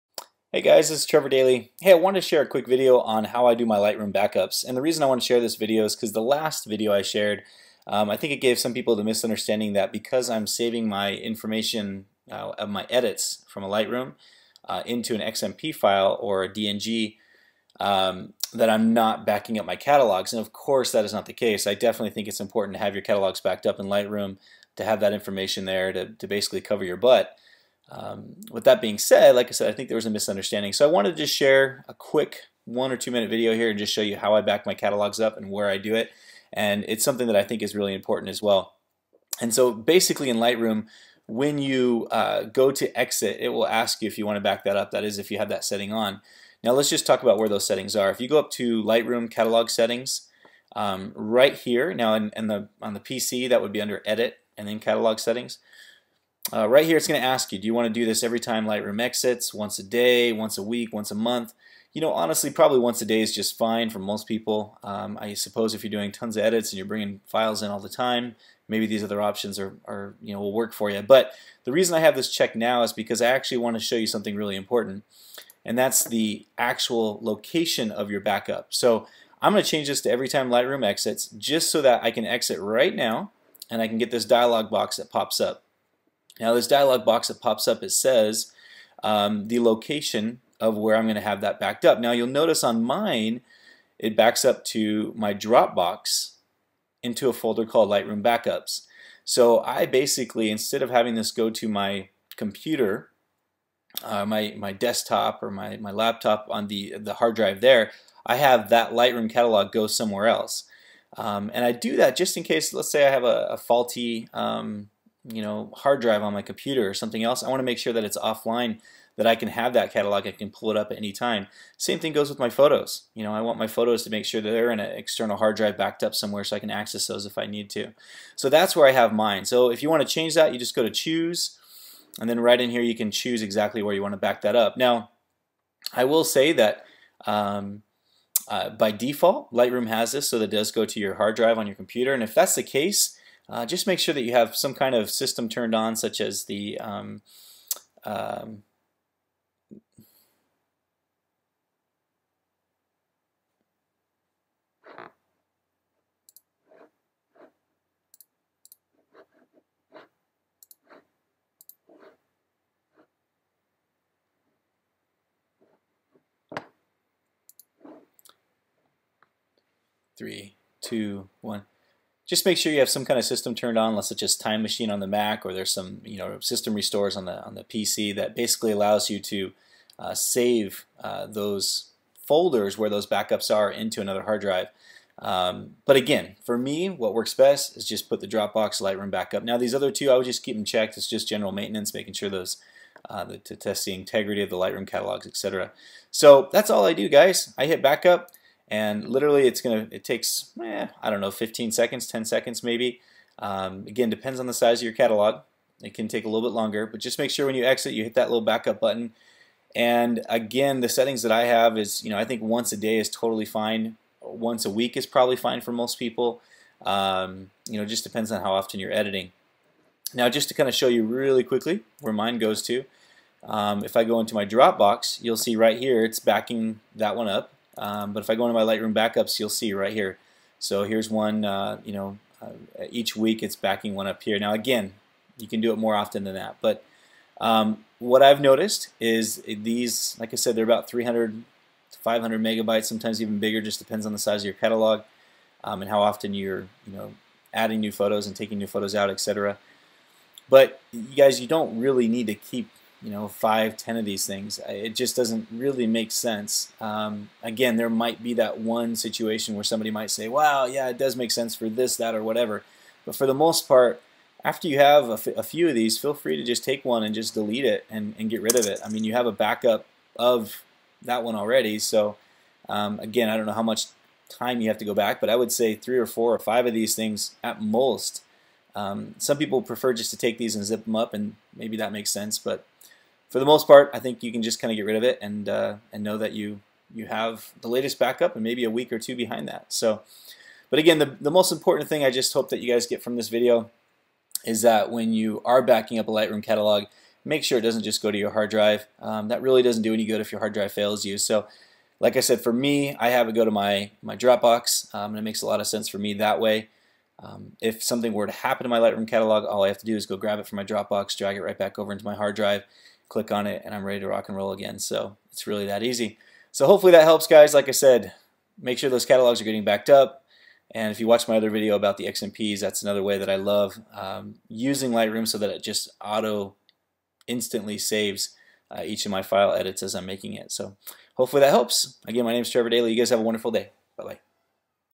Backing Up Your Lightroom Catalog in the Cloud
It is very important to back up your Lightroom catalogs - but make sure that when you do it you change the default setting on Lightroom so it doesn't just save on the same hard drive where your working catalog is being saved as well.